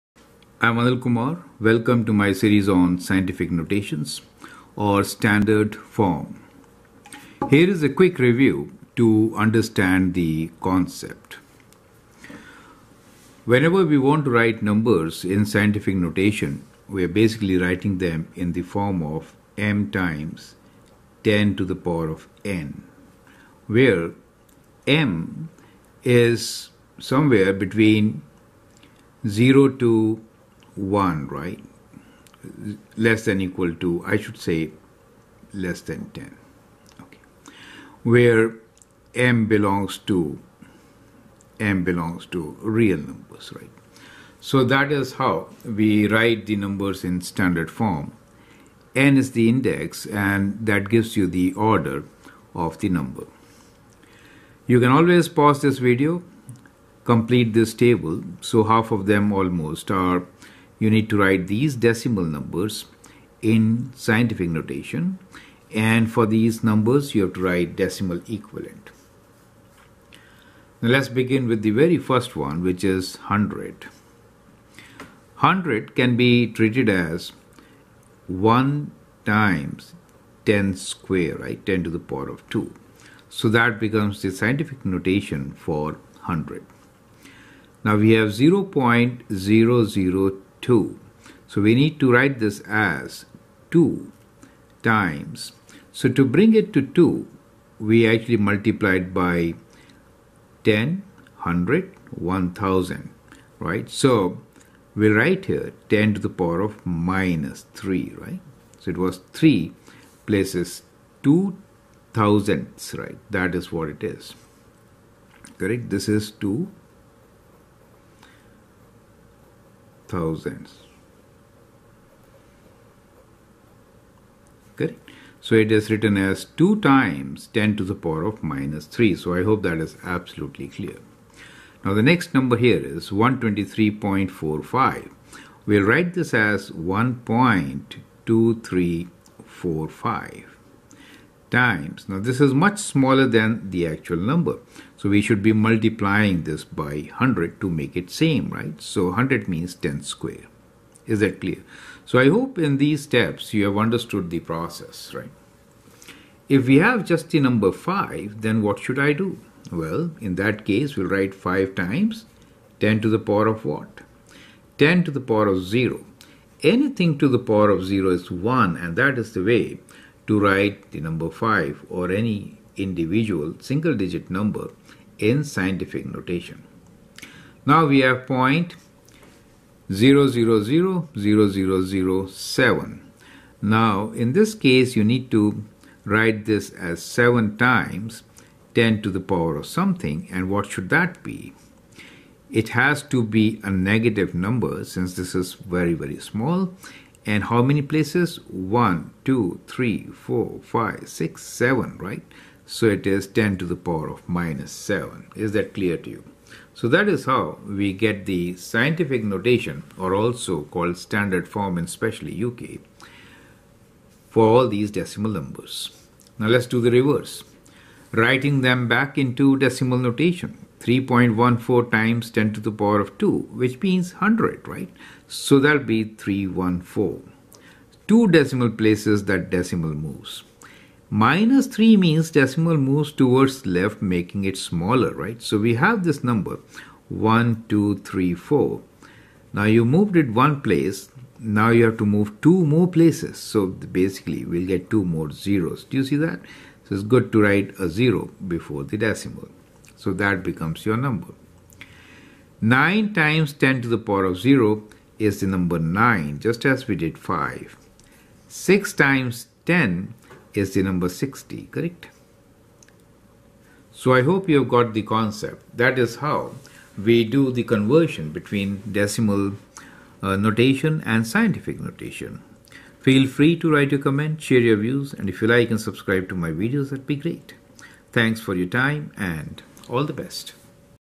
How to Write Decimal Numbers in Standard Form or Scientific Notation
Scientific Notation: Writing real numbers that are too large or too small in decimal form as a×10^n where absolute value of a is at least 1 but less than 10 and the exponent of 10 is an integer.
The coefficient a is called the significand. It can have negative values. The exponent n is called order of magnitude. It helps to compare numbers easily.